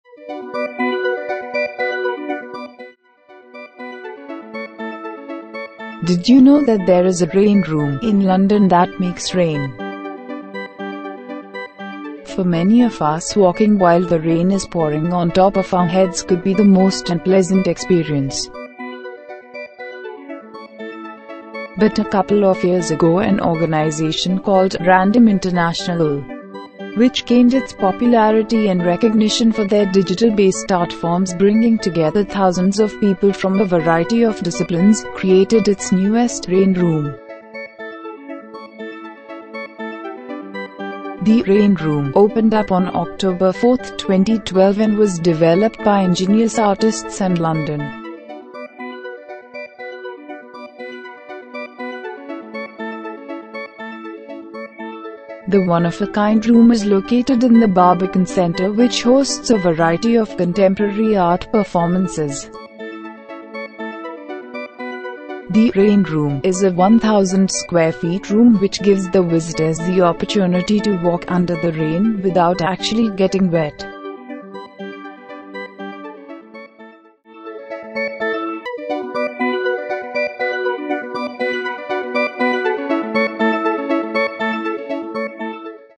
Artificial Rain in London “Rain Room” - Nutshell School
Artificial Rain – Did You Know That There Is A “Rain Room” In London That Makes Rain. It Is The Most Amazing Place In The World.

For many of us walking while rain is pouring on top of our heads could be the most unpleasant experience. But a couple of years ago an organization called “Random International” Which gained its popularity and recognition for their digital-based art forms bringing together thousands of people from a variety of disciplines, created its newest “rain room”.

The one of a kind room is located in the barbican centre which hosts a variety of contemporary art performances.

The “Rain Room” opened up on October 4, 2012 and was developed by ingenious artists in London. The rain room is a 1000 square feet room which the Visitors the opportunity to walk under the Artificial Rain without actually getting wet.

Rain Room allows visitors to the installation to walk through a downpour without getting wet. Motion sensors detect visitors’ movements as they navigate through the darkened space. Becoming “performers in this intersection of art, technology and nature”.

Artificial Rain
Rain Room is a 2012 experiential artwork by Hannes Koch and Florian Ortkrass for Random Internationals , which found its first permanent installation in Sharjah , United Arab Emirates in 2018.

Previos Installation

Kate Bush (curator), Head of Art Galleries, Barbican Centre, said: “The Curve has previously played host to guitar-playing finches, a World War II bunker and a digital bowling alley. Random International have created a new work every bit as audacious and compelling ? Rain Room surpasses all our expectations.

Nutshell School Tags – england installation artificial falling water design contemporary artificial rain in london sharjah united arab emirates